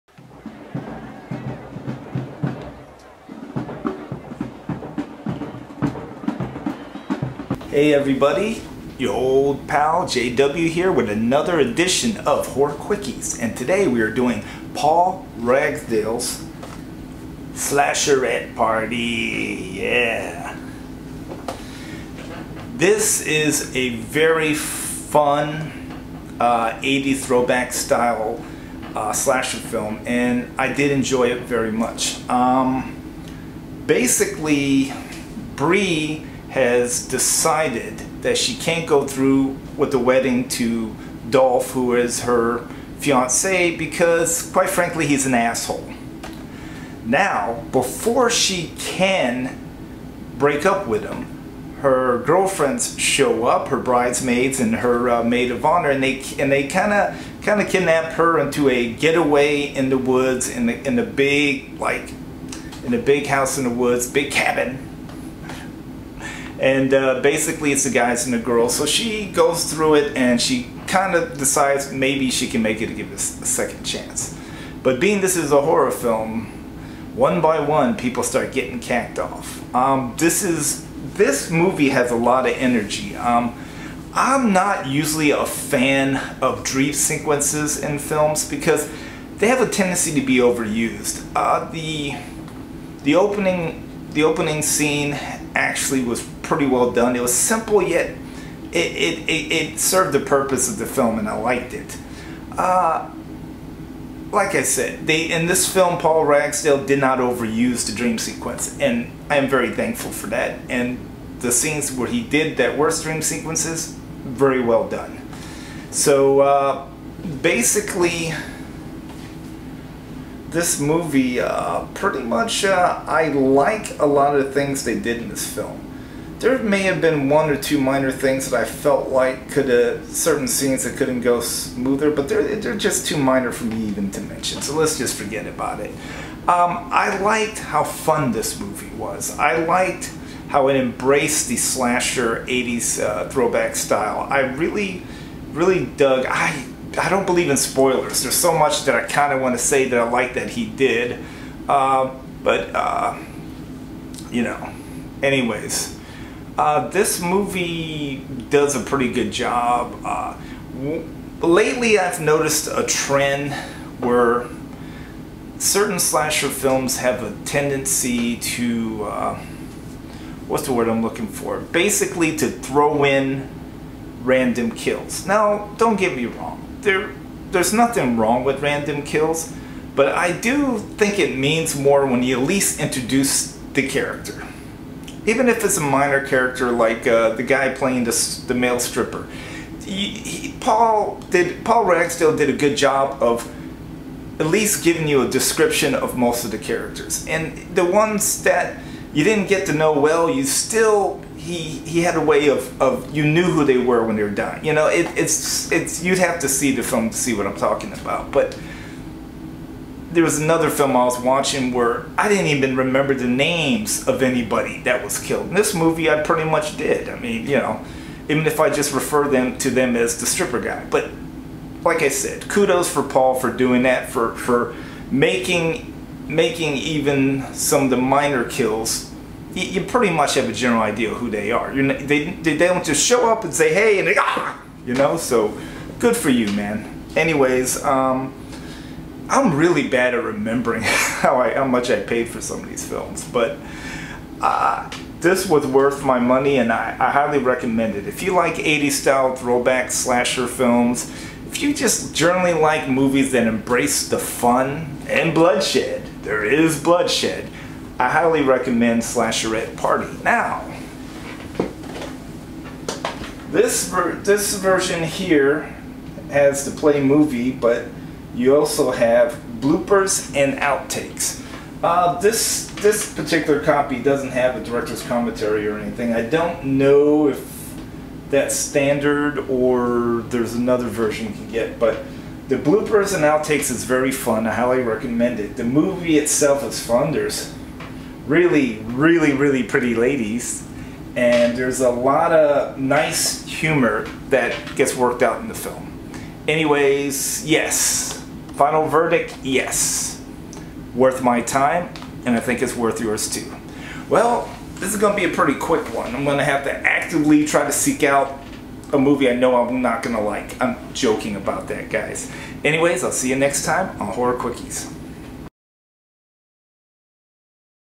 Horror Quickies: Slashlorette Party
JW reviews Slashorette Party!